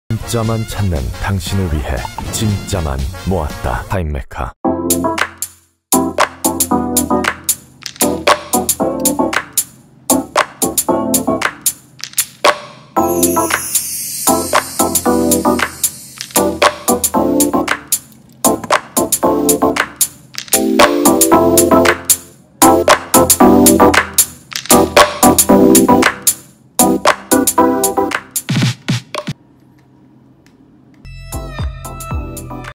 시티즌 AN8170-59E 메탈시계 리뷰 영상 - 타임메카
시티즌 AN8170-59E 메탈시계 리뷰 영상

Movement : Quartz / 쿼츠
Case : Stainless Steel / 스테인레스 스틸
Band : Stainless Steel / 스테인레스 스틸
Water-Resistance(방수) : 10ATM / 100Meters
Case-Size(지름) : 40mm x 40mm
Function(기능) : Hours / 시# Minutes / 분# Seconds / 초# Date / 날짜# Chronograph / 크로노그래프